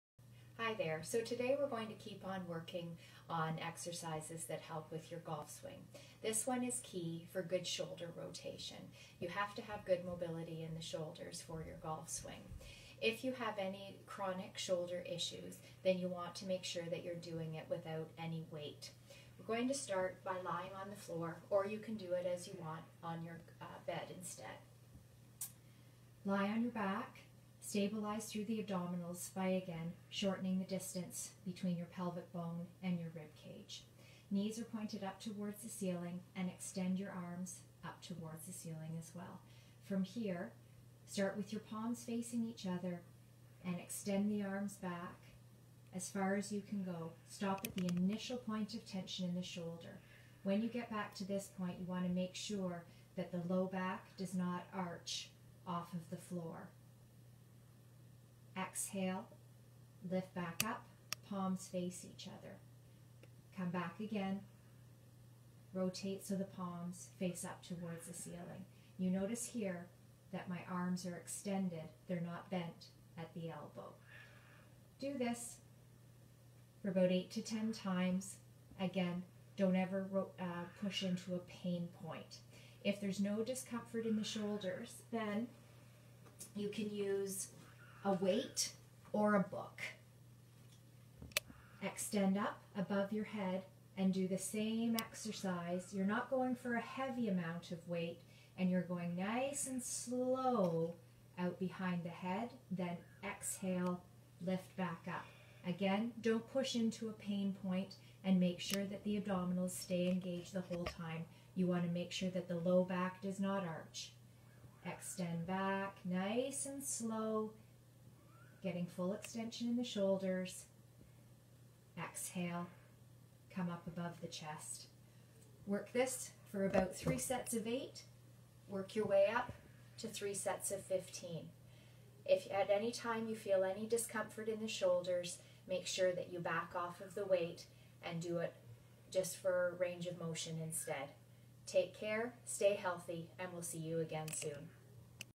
Pullover for Shoulder Rotation | RiverBend Golf Community
Use this pullover exercise to help with shoulder rotation in your golf swing!